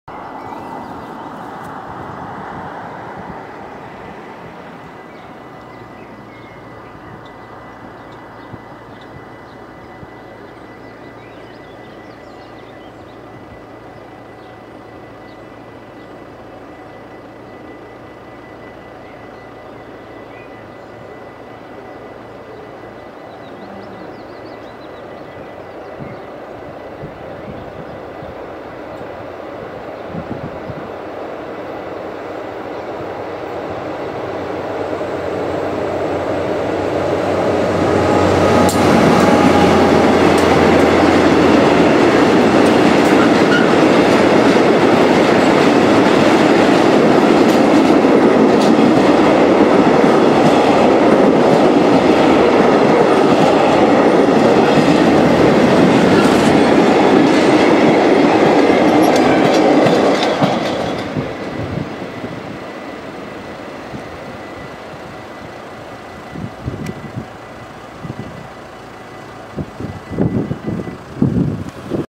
DBSRN 6414 komt met UC door Glimmen. 03-06-2010
Vanavond was ik nog even wezen kijken bij Glimmen. En daar kwam de 6414 met Unit Cargo naar Kijfhoek weer voorbij. De trein bestond uit 8 ketelwagens, 5 rongenwagens en 3 schuifwandwagens. Normaal had ik hem ook nog wel op foto gezet, maar op het ogenblik is gras zo hoog op deze lokatie dat het knap lastig word om de trein goed op de plaat te krijgen.